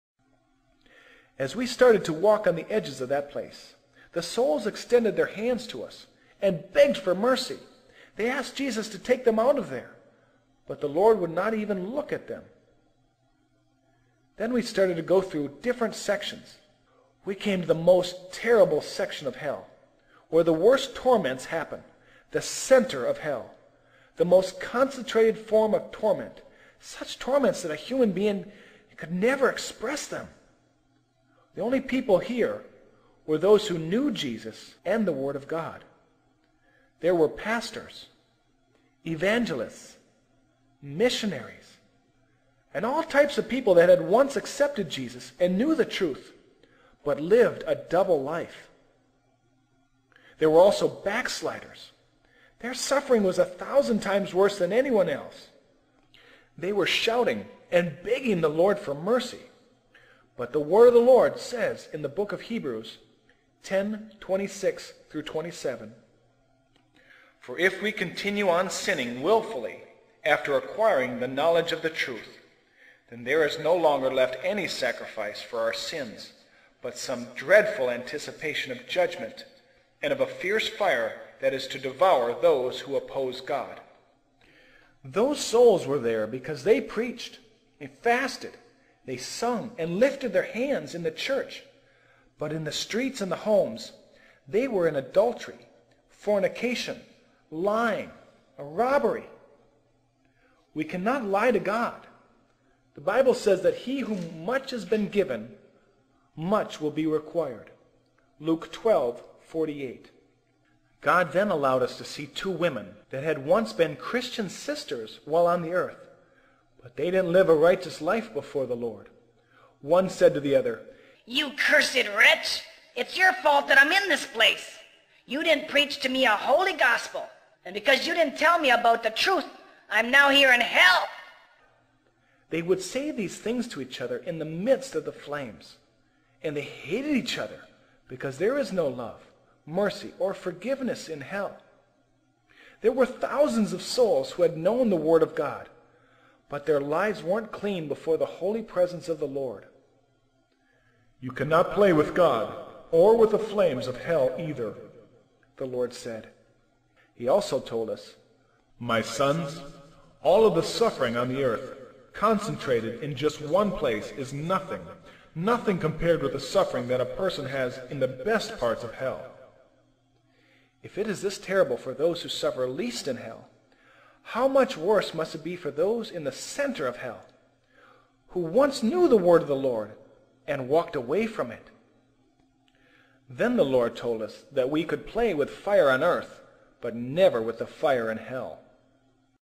Punishment In The Center Of Hell For Double Life
I have backslidden myself! This short video is the consequences for those who have tasted that the Lord is Good and have walked away, and have back slidden into the world will receive the most punishment as they had the life and trampled Jesus under their feet. It's like Jesus says about Judas, it would of been best if he were never born! Please come back to the Lord and if you never accepted Him please ask that He come into your life, and ask in Faith, believing that He is listening, because their is no turning back! Please be saved! Give HIM A CHANCE AND YOURSELF!